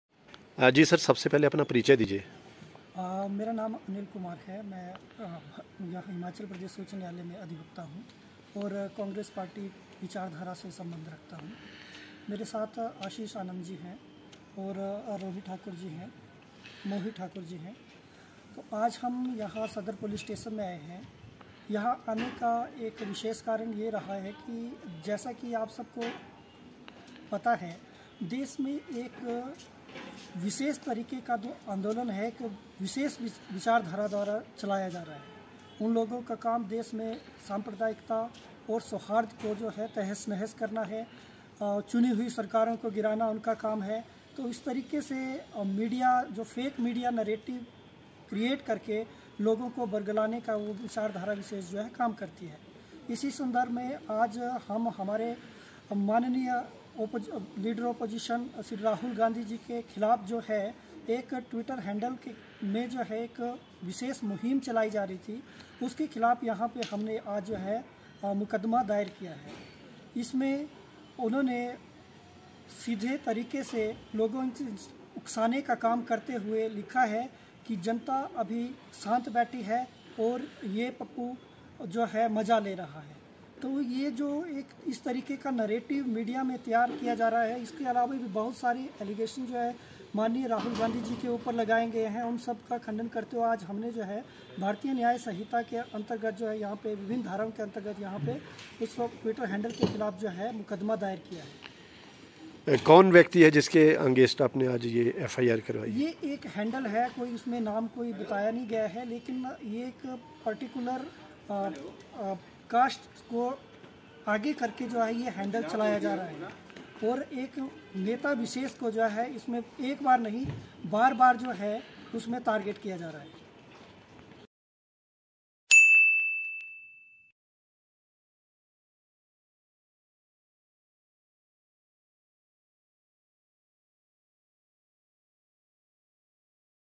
Shimla | राहुल गांधी के खिलाफ दुष्प्रचार पर शिमला सदर थाना में कांग्रेस लीगल सेल ने की शिकायत
शिमला : राहुल गांधी के खिलाफ दुष्प्रचार पर शिमला सदर थाना में कांग्रेस लीगल सेल ने की शिकायत, कार्यवाई की उठाई मांग
Shimla: Congress legal cell lodged complaint in Shimla Sadar police station on false propaganda against Rahul Gandhi, raised demand for action.

Keep Sharing

Keep Supporting